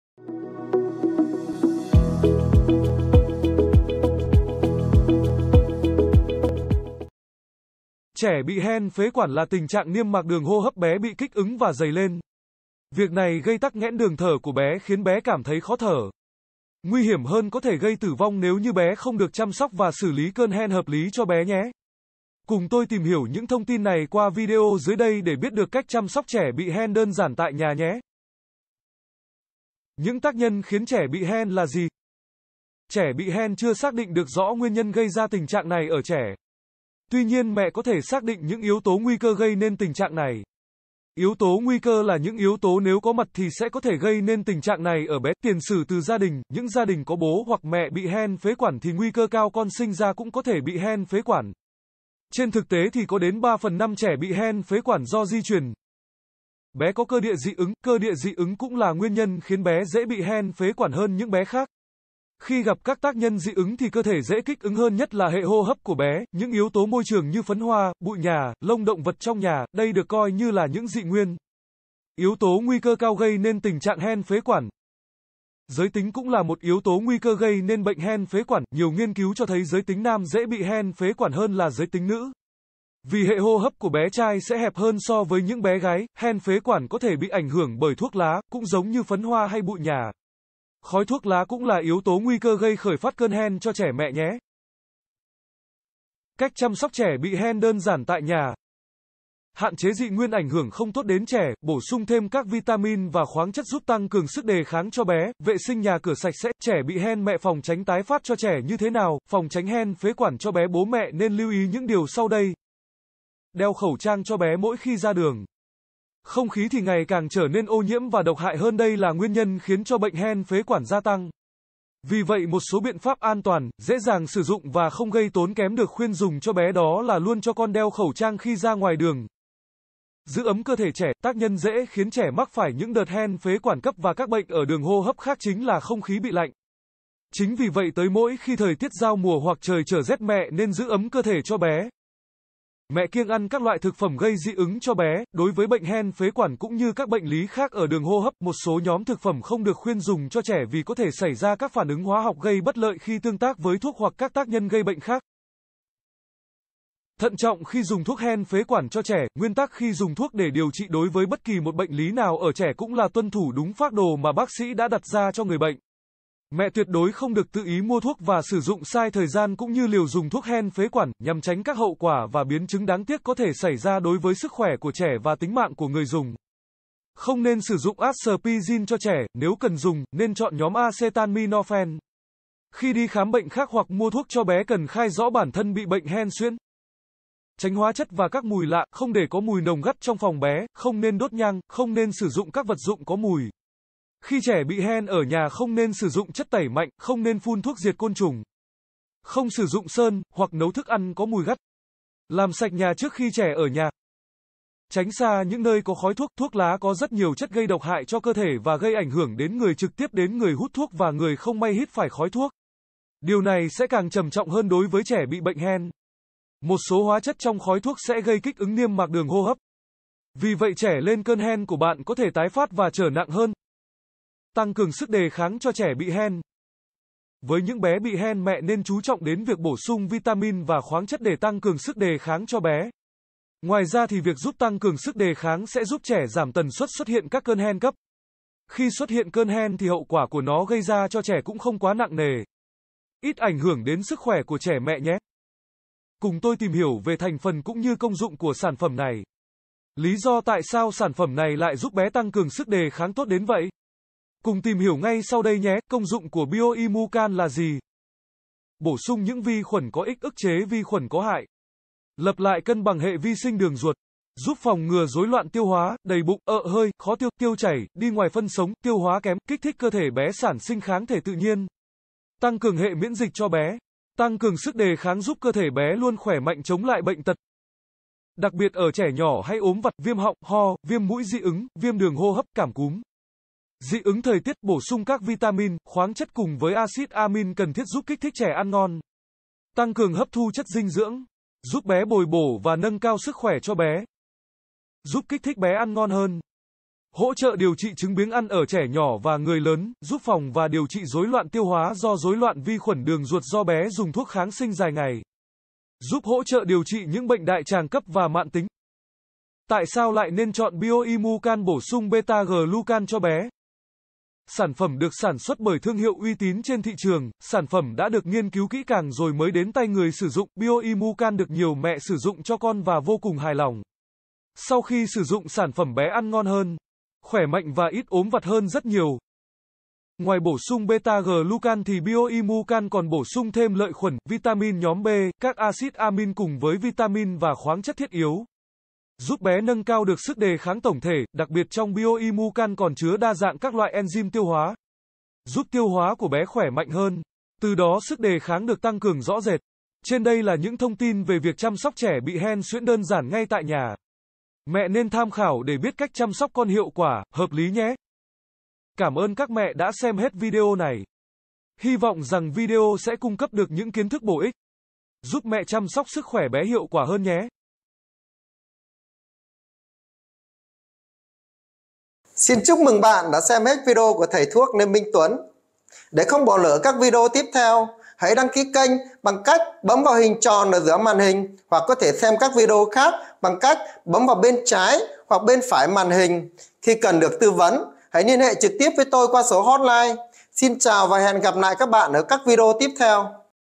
Chăm sóc trẻ bị hen đơn giản bằng những thứ có sẵn trong nhà
Những tác nhân khiến trẻ bị hen là gì?
Trẻ bị hen chưa xác định được rõ nguyên nhân gây ra tình trạng này ở trẻ. Tuy nhiên mẹ có thể xác định những yếu tố nguy cơ gây nên tình trạng này. Yếu tố nguy cơ là những yếu tố nếu có mặt thì sẽ có thể gây nên tình trạng này ở bé.
Tiền sử từ gia đình: Những gia đình có bố hoặc mẹ bị hen phế quản thì nguy cơ cao con sinh ra cũng có thể bị hen phế quản. Trên thực tế thì có đến ⅗ trẻ bị hen phế quản do di truyền.
Bé có cơ địa dị ứng: Cơ địa dị ứng cũng là nguyên nhân khiến bé dễ bị hen phế quản hơn những bé khác. Khi gặp các tác nhân dị ứng thì cơ thể dễ kích ứng hơn nhất là hệ hô hấp của bé.
Những yếu tố môi trường như phấn hoa, bụi nhà, lông động vật trong nhà: Đây được coi như là những dị nguyên. Yếu tố nguy cơ cao gây nên tình  trạng hen phế quản.
Giới tính cũng là một yếu tố nguy cơ gây nên bệnh hen phế quản: Nhiều nghiên cứu cho thấy giới tính nam dễ bị hen phế quản hơn là giới tính nữ. Vì hệ hô hấp của bé trai sẽ hẹp hơn so với những bé gái.
Hen phế quản có thể bị ảnh hưởng bởi thuốc lá: Cũng giống như phấn hoa hay bụi nhà. Khói thuốc lá cũng là yếu tố nguy cơ gây khởi phát cơn hen cho trẻ mẹ nhé.
Cách chăm sóc trẻ bị hen đơn giản tại nhà

Trẻ bị hen phế quản mẹ nên chăm sóc bé:

Hạn chế dị nguyên ảnh hưởng không tốt đến trẻ.
Bổ sung thêm các vitamin và khoáng chất giúp tăng cường sức đề kháng cho bé.
Vệ sinh nhà cửa sạch sẽ.
Trẻ bị hen mẹ phòng tránh tái phát cho trẻ như thế nào
Phòng tránh hen phế quản cho bé bố mẹ nên lưu ý những điều sau đây:

Đeo khẩu trang cho bé mỗi khi ra đường: Không khí thì ngày càng trở nên ô nhiễm và độc hại hơn đây là nguyên nhân khiến cho bệnh hen phế quản gia tăng. Vì vậy một số biện pháp an toàn, dễ dàng sử dụng và không gây tốn kém được khuyên dùng cho bé đó là luôn cho con đeo khẩu trang khi ra ngoài đường.
Giữ ấm cơ thể trẻ: Tác nhân dễ khiến trẻ mắc phải những đợt hen phế quản cấp và các bệnh ở đường hô hấp khác chính là không khí bị lạnh. Chính vì vậy tới mỗi khi thời tiết giao mùa hoặc trời trở rét mẹ nên giữ ấm cơ thể cho bé.
Mẹ kiêng ăn các loại thực phẩm gây dị ứng cho bé: Đối với bệnh hen phế quản cũng như các bệnh lý khác ở đường hô hấp, một số nhóm thực phẩm không được khuyên dùng cho trẻ vì có thể xảy ra các phản ứng hóa học gây bất lợi khi tương tác với thuốc hoặc các tác nhân gây bệnh khác.
Thận trọng khi dùng thuốc hen phế quản cho trẻ: Nguyên tắc khi dùng thuốc để điều trị đối với bất kỳ một bệnh lý nào ở trẻ cũng là tuân thủ đúng phác đồ mà bác sĩ đã đặt ra cho người bệnh. Mẹ tuyệt đối không được tự ý mua thuốc và sử dụng sai thời gian cũng như liều dùng thuốc hen phế quản, nhằm tránh các hậu quả và biến chứng đáng tiếc có thể xảy ra đối với sức khỏe của trẻ và tính mạng của người dùng. Không nên sử dụng Aspirin cho trẻ; Nếu cần dùng, nên chọn nhóm Acetaminophen. Khi đi khám bệnh khác hoặc mua thuốc cho bé cần khai rõ bản thân bị bệnh hen suyễn.
Tránh hóa chất và các mùi lạ: Không để có mùi nồng gắt trong phòng bé, Không nên đốt nhang, Không nên sử dụng các vật dụng có mùi. Khi trẻ bị hen ở nhà không nên sử dụng chất tẩy mạnh, không nên phun thuốc diệt côn trùng, không sử dụng sơn, hoặc nấu thức ăn có mùi gắt. Làm sạch nhà trước khi trẻ ở nhà.
Tránh xa những nơi có khói thuốc: Thuốc lá có rất nhiều chất gây độc hại cho cơ thể và gây ảnh hưởng đến người trực tiếp đến người hút thuốc và người không may hít phải khói thuốc. Điều này sẽ càng trầm trọng hơn đối với trẻ bị bệnh hen. Một số hóa chất trong khói thuốc sẽ gây kích ứng niêm mạc đường hô hấp, vì vậy trẻ lên cơn hen của bạn có thể tái phát và trở nặng hơn.